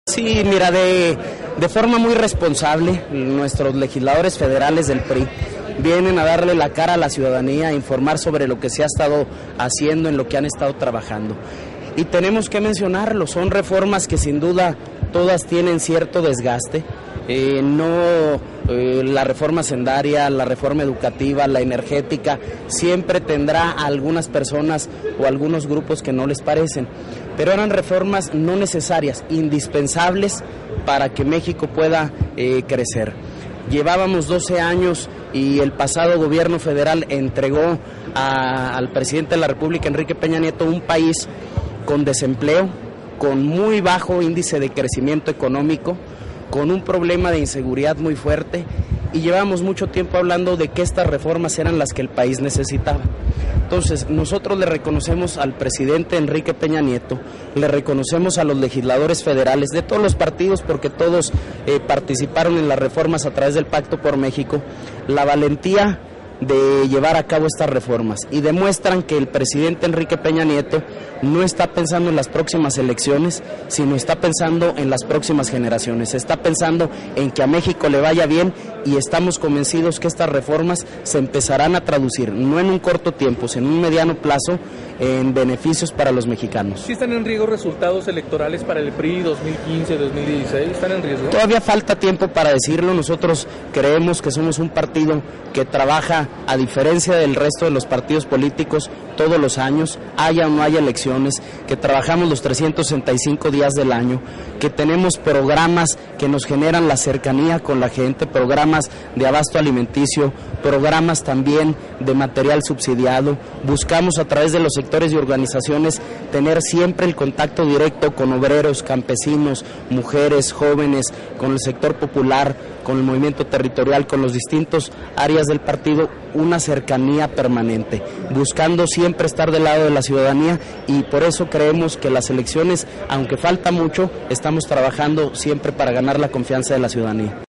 Otniel García, Legisladores Federales vienen a dar la cara a la ciudadanía
Presidente del comité directivo estatal del PRI comenta que los  legisladores federales vienen a darle la cara a la ciudadanía de los trabajos que se han realizado y de las reformas que se han aprobado en el congreso de la que tienen cierto desgaste, así mismo sobre los beneficios que traerán estás a todo México